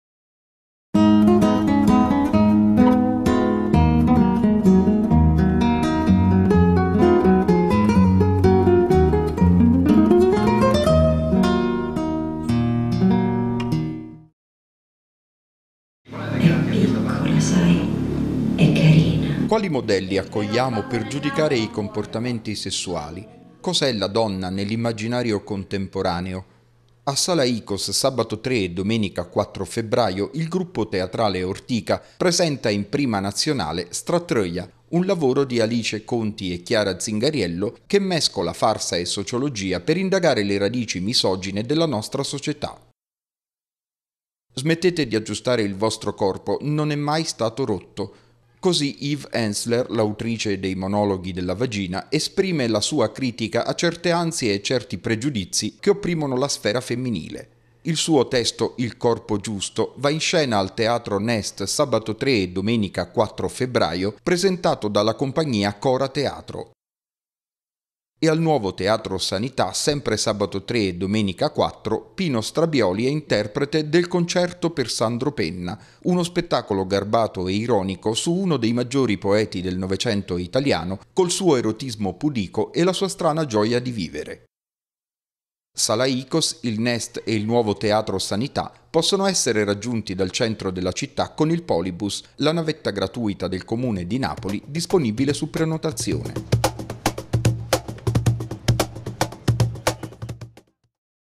Indovina chi viene in scena. A teatro col Polibus (2 febbraio 2018)
Rubrica di proposte teatrali per il fine settimana, con uno sguardo ai teatri del centro e della periferia raggiunti dal Polibus, la speciale navetta del Comune di Napoli che accompagna gratuitamente gli spettatori a teatro.